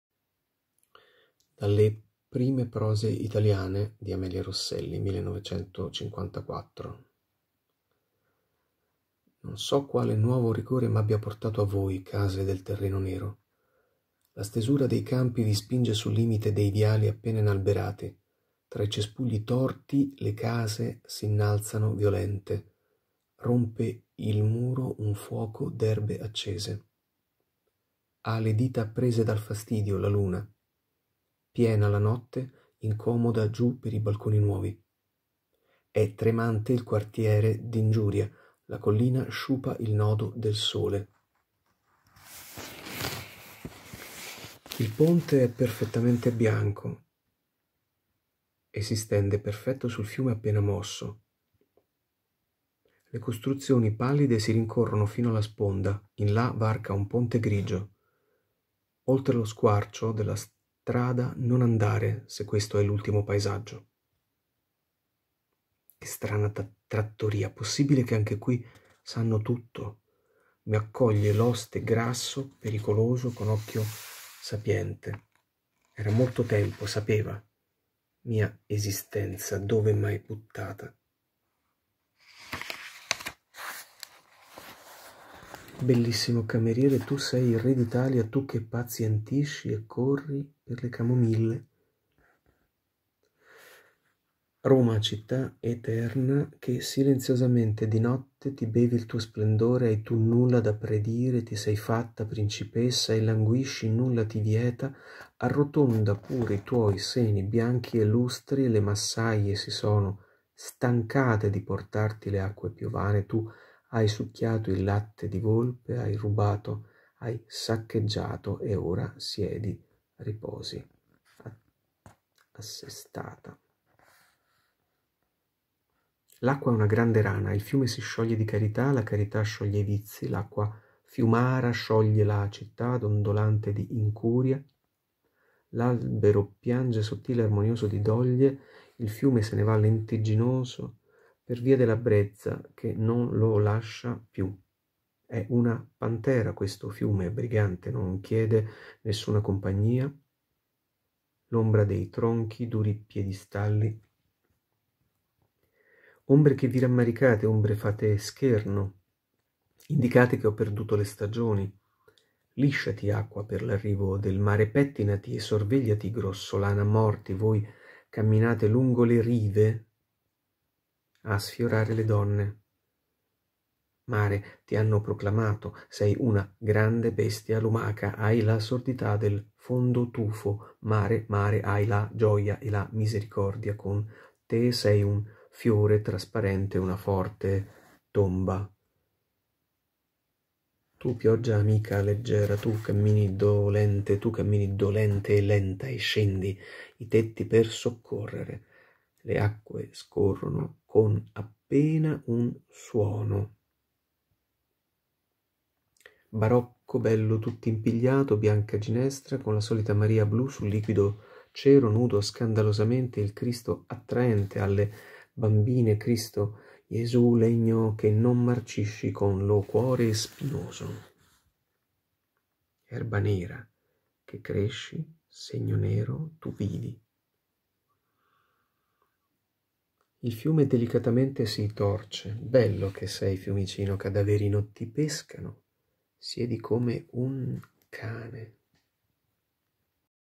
Lettura di testi dalle "Prime prose italiane" di Amelia Rosselli (1954)
MG legge alcuni testi dalle "Prime prose italiane" di Amelia Rosselli (1954). Registrazione audio/video del novembre 2020